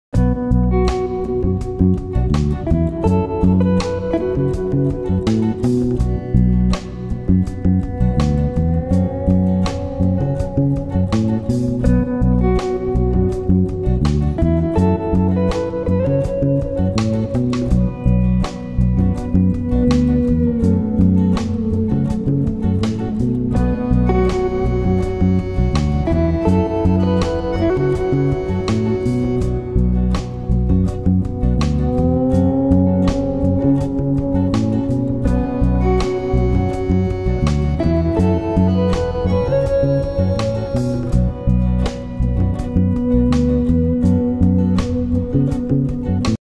Festive pump Black Dresses Boutique jurkenwinkel Sittard
Feestelijke pump met glittertje. De hakhoogte is 8 cm.